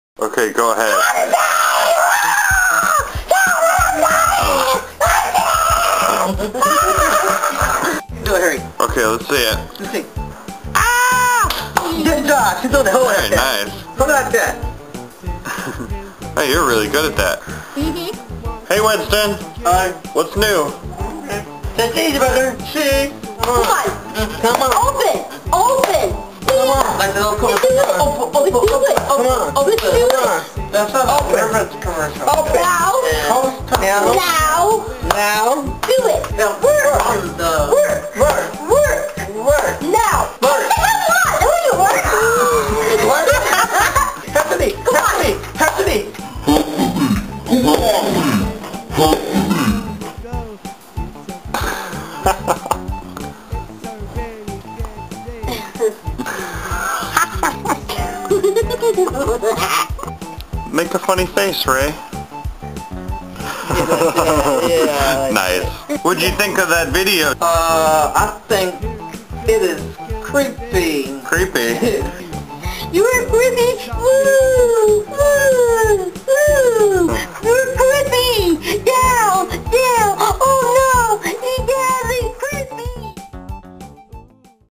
Revenge of the Bloopers
The semester is coming to close, here is a short video of a few of the silly moments from our class.